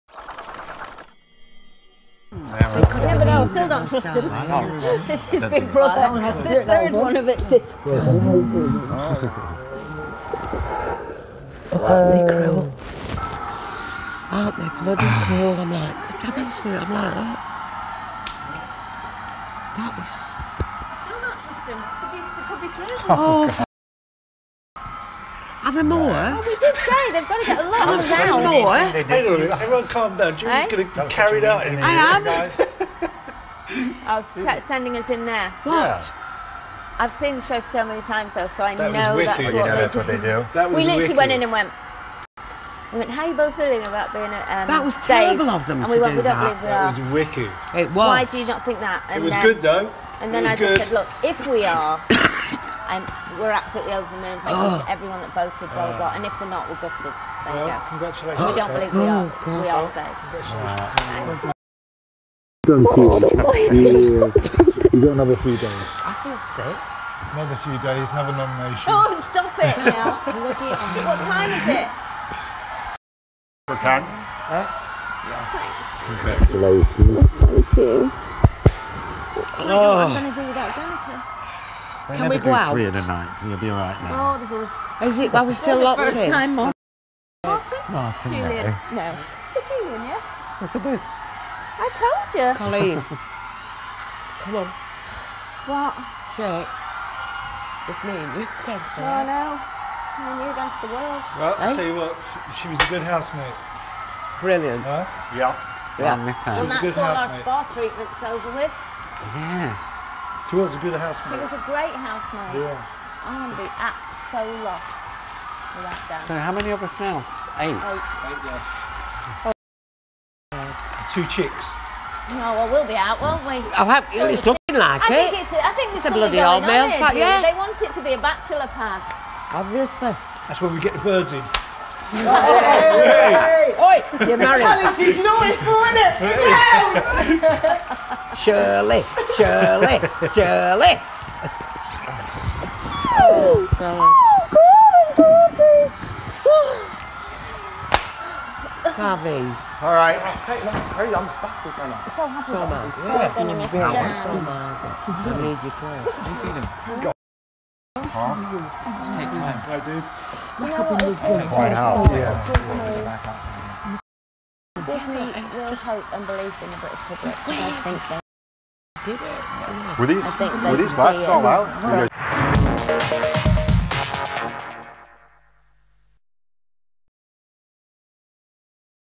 The House react to Danicas eviction
The House react to Danica being the next celebrity to be evicted. Julie and Coleen congratulate themselves for being the only females left in the House.